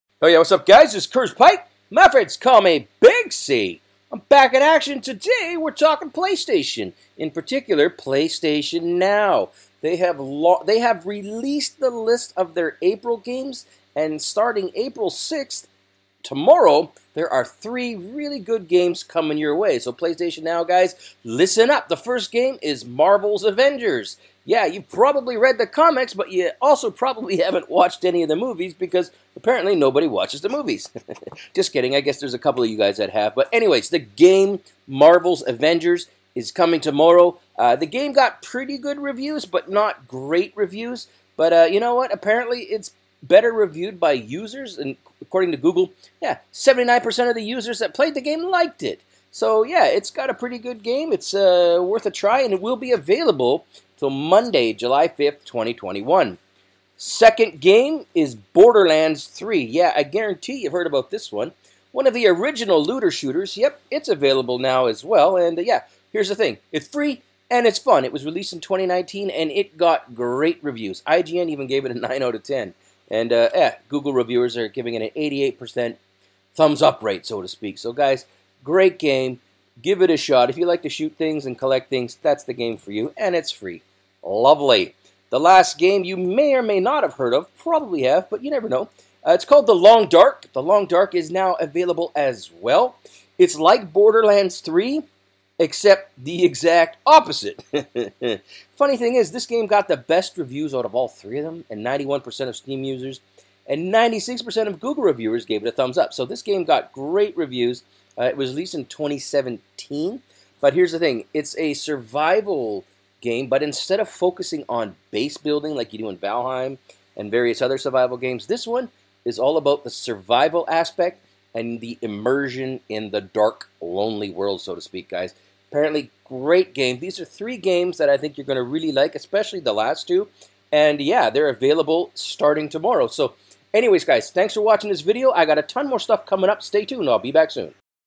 Games for PS NOW - April 2021 - MGN TV
Playstation Now, aka PS NOW,  has released their April 20201 games.  Starting April 6th, 3 big games coming your way.

Marvel’s Avengers

First up is Marvel’s Avengers.  It released in August of 2019.  The game got decent, but not great reviews.  Appears gamers liked it al little more than the critics - with 79% of google users liking the game.

Marvel’s Avengers is available until Monday July 5, 2021.

Borderlands 3

Lots of talk, good and bad, lately about Outriders - but let’s not forget about one of the original looter shooters - Borderlands.  Well, Borderlands 3 is free and it’s a lot of fun.  Released in August of 2019, it got great reviews, with IGN giving it a 9/10.  88% of Google reviewers enjoyed the game, it’s just good.

Borderlands 3 is available until Wednesday September 29, 2021.

This game is like the EXACT opposite of Borderlands 3, and it is the highest reviewed game of the three.  91% of Steam users like the game and 96% of Google reviewers gave it the thumbs up.  Instead of focusing on base building like in Valheim, this game is all about the survival aspect and your immersion in a dark, lonely world.

Great games for April, Playstation owners and PS Now subscribers should be pleased!